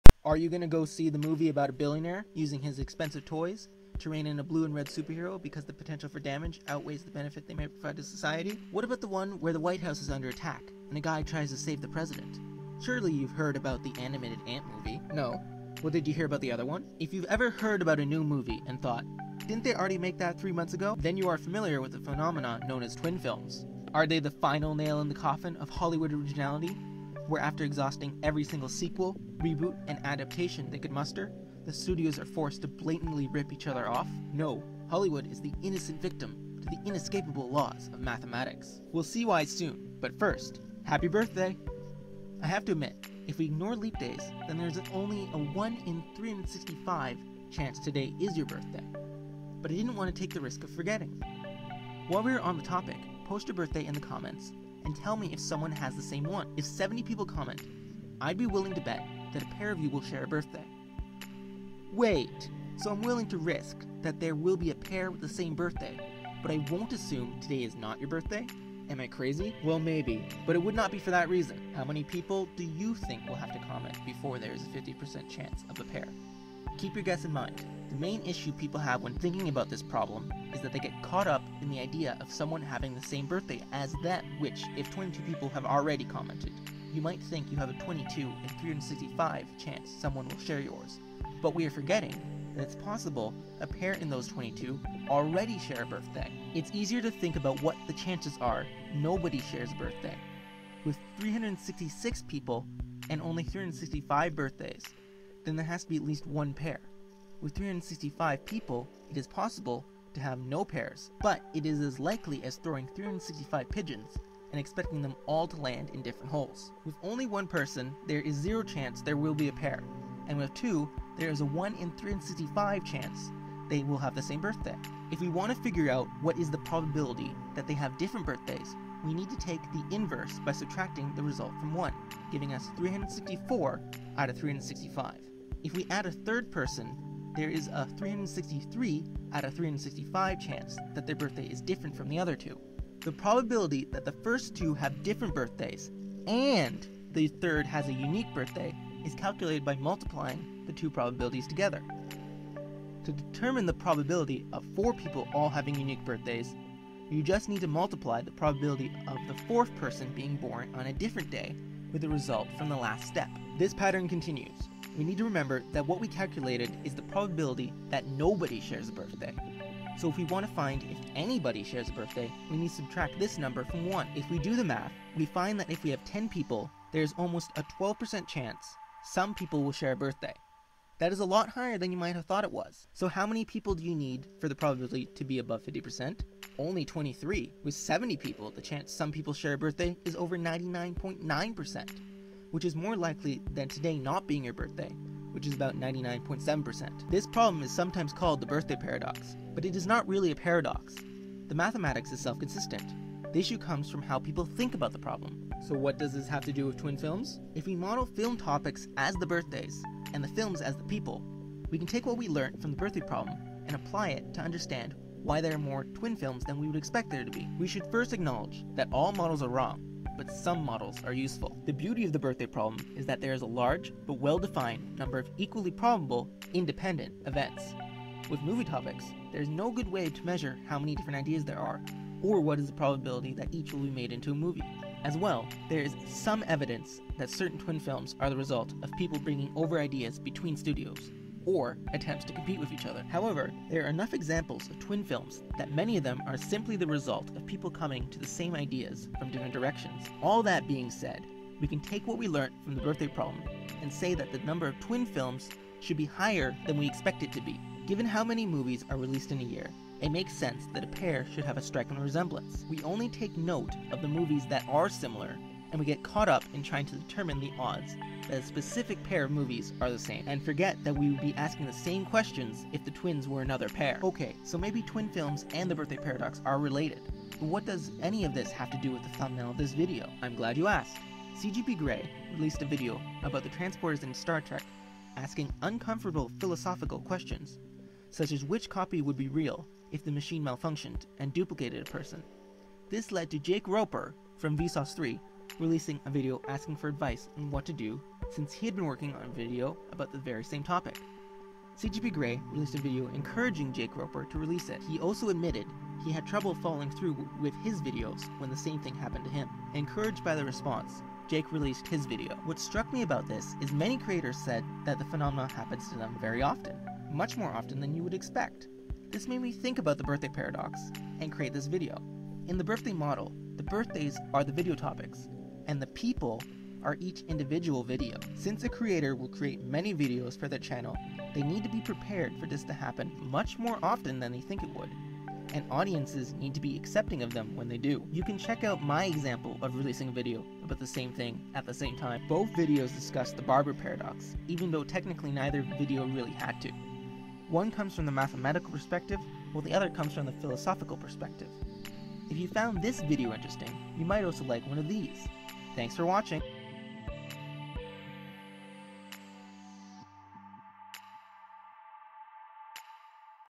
Why Twin Films Exist Explained with Math (Birthday Paradox)
Have you ever wondered why Hollywood sometimes releases movies with very similar plots at the same time? Finally an explanation!

Sometimes there are movies with the same plot released at the same time. These include Batman V Superman: Dawn of Justice and Captain America: Civil War, which both have superheroes fighting. But there are also many examples such as White House Down and Olympus has Fallen. A Bug's Life and Antz. Armageddon and Deep Impact, and the list goes on.

Is this just Hollywood copying each other? While sometimes this is the case, sometimes espionage is the case scroll to Feud between DreamWorks and Pixar), or the movies are released intenionally close to each other to compete: (Captain America: Civil War was originally going to be released on the same day as Batman V Superman, this forced the DC films to be released earlier than intended to avoid competing directly), but sometimes they are completely innocent cases of two people coming up with the same idea The top comment has 4 links which explain why the two movies about the white house being attacked did not copy one another)

So why does it happen so often?

Because of the Birthday Paradox.

In short humans are bad at predicting how likely collisions between two things occur when they can collide with many different things at the same time.

You can watch part 2 here, where we assume Hollywood is guilty use Game Theory to analyse why studios might want to copy each other, and the Nash Equilibrium means they often do even if they would be better off not copying.

A list of twin films, also called Copy Cat Movies:

This is a list that appeared in the video in some way

Batman V Superman | Marvel Civil War
The Jungle Book (Disney) | Jungle Book(2018)(Warner Brothers)
Olympus Has Fallen | White House Down
No Strings Attached | Friends With Benefits
Armageddon | Deep Impact
A Bug's Life | Antz
Volcano | Dante's Peak
After Earth | Oblivion
Babe | Gordy
Robin Hood | Robin Hood: Prince of Thieves
Terminal Velocity | Dropzone
Jaws | Orca
Jurassic Park | Carnosaur

Here are the videos by CGP Grey and VSauce3 that I discussed